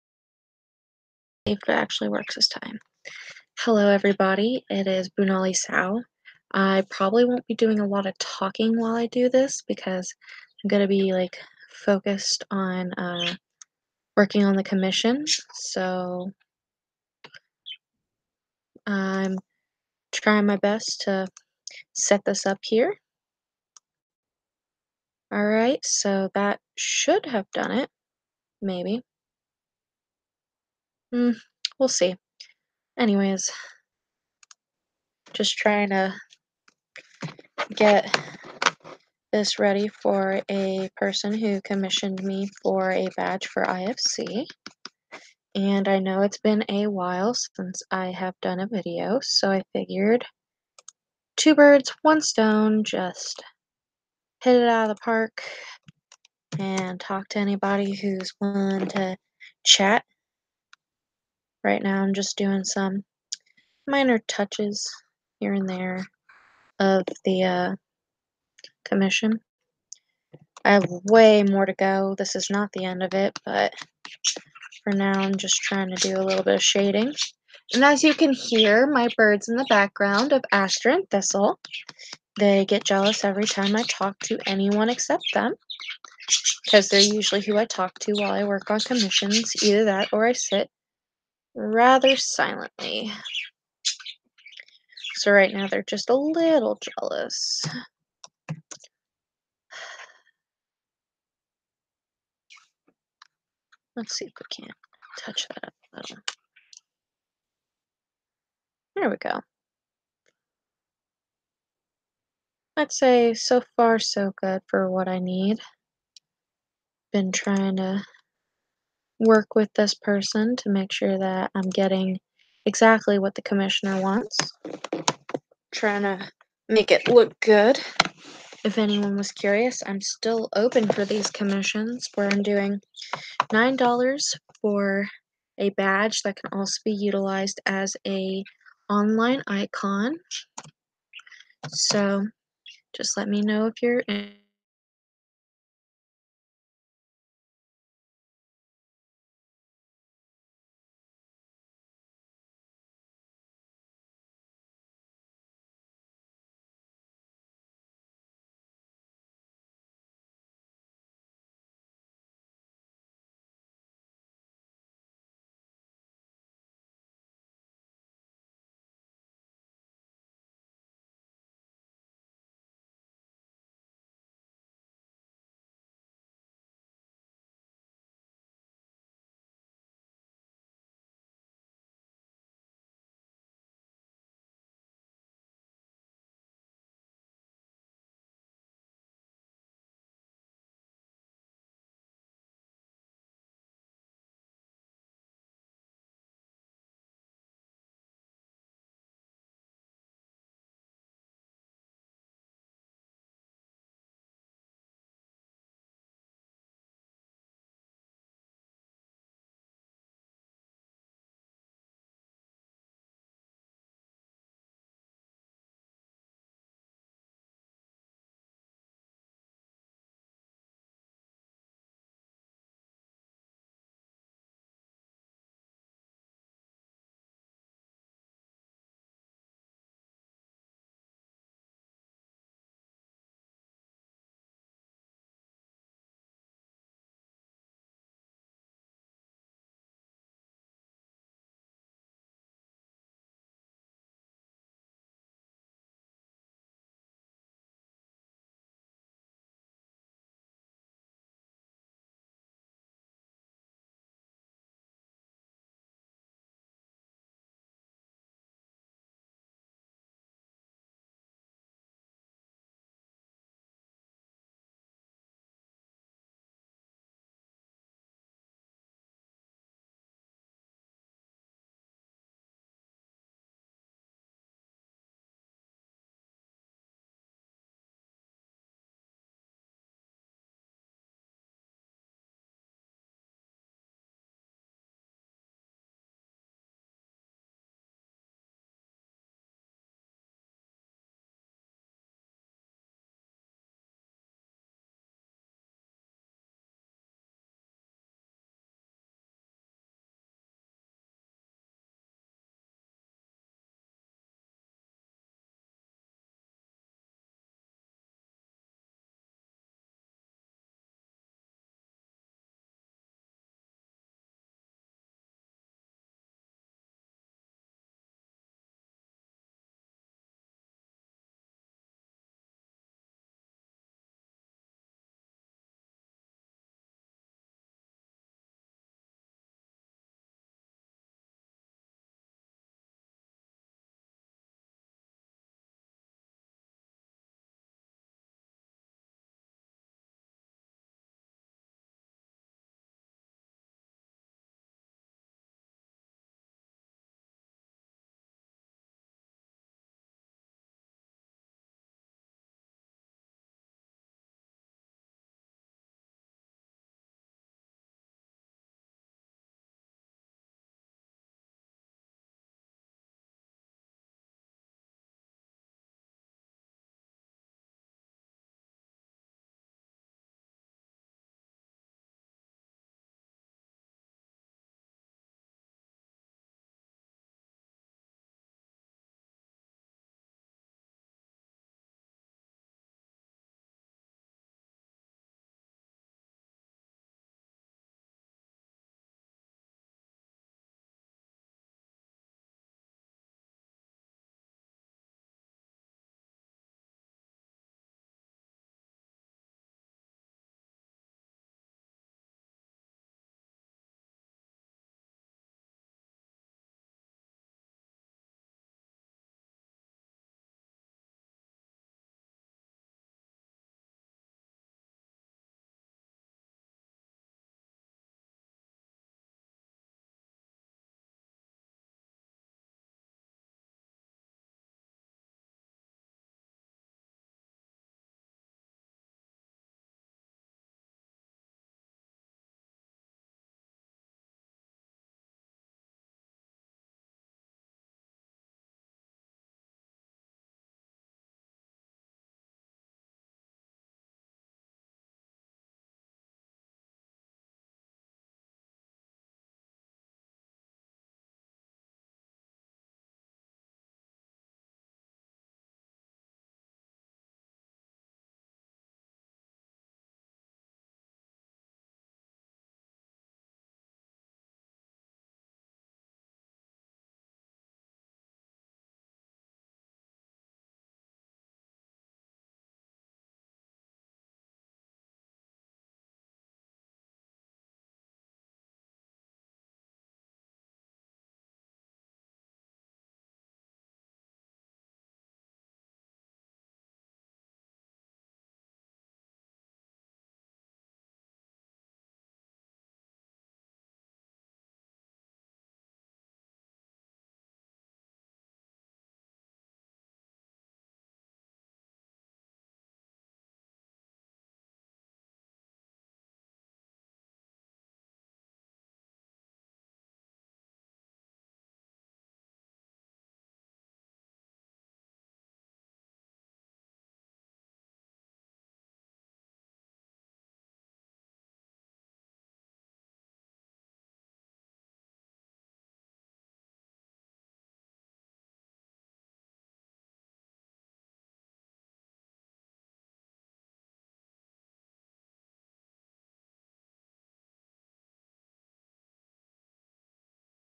Benji Analog Dog --- Hawaii' FC (Indy Fur Con)
Commission for a badge.

For Indy Fur Con with their Hawaii theme.

Border Collie fursona with a 90's summer vibe.